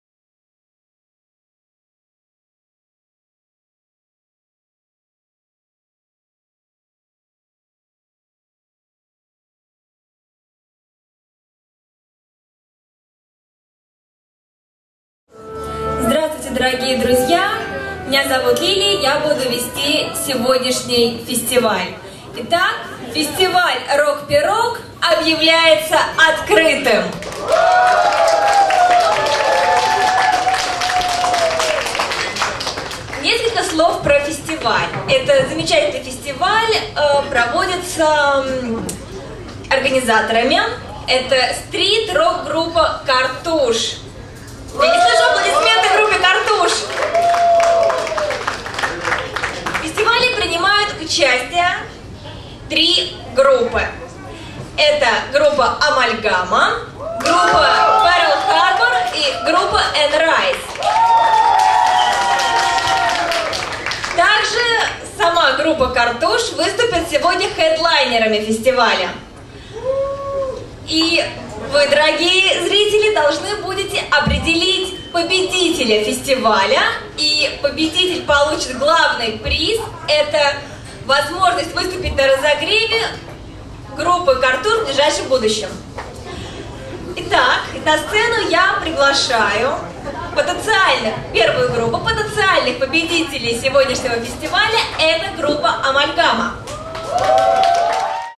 Открытие фестиваля "Рок-пирог"
Живое ТВ представляет трансляцию Фестиваля "Рок-пирог" 28 мая 2008 года в клубе "Меццо-forte"